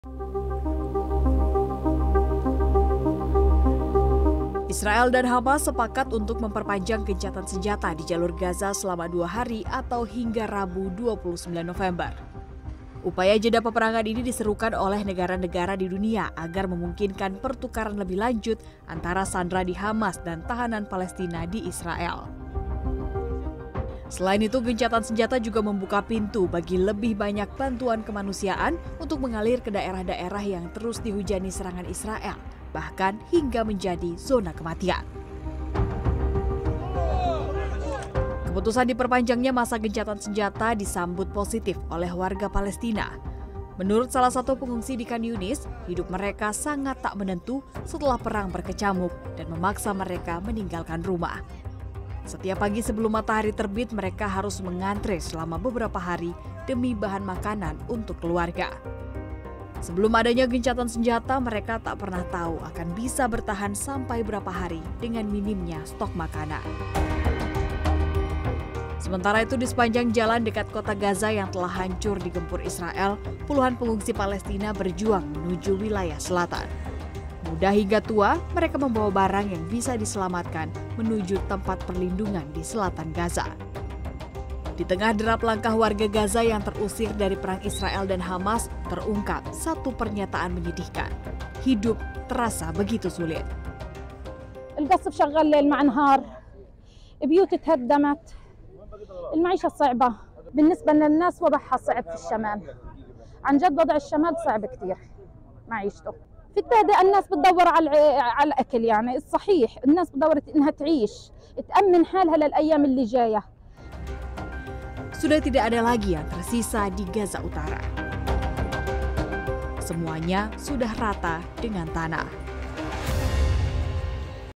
Gencatan Senjata Israel Hamas Diperpanjang 2 Hari, Pengungsi Sambut Baik
Gencatan senjata antara Israel dan Hamas, resmi diperpanjang dua hari, dari yang sebelumnya berakhir pada senin.

Keputusan diperpanjangnya masa gencatan senjata, disambut positif oleh warga Palestina.